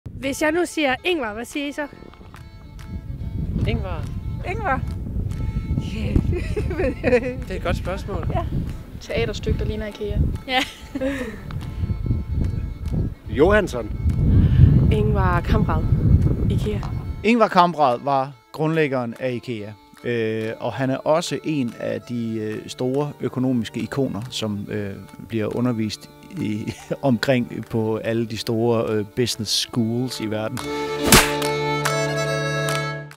INGVAR!: Hvis jeg siger Ingvar, hvad siger du så?
Vi gik en tur i Fredericia og spurgte indbyggerne hvad de tænkte på, hvis vi sagde INGVAR!.

Filmet af Emilie Vinther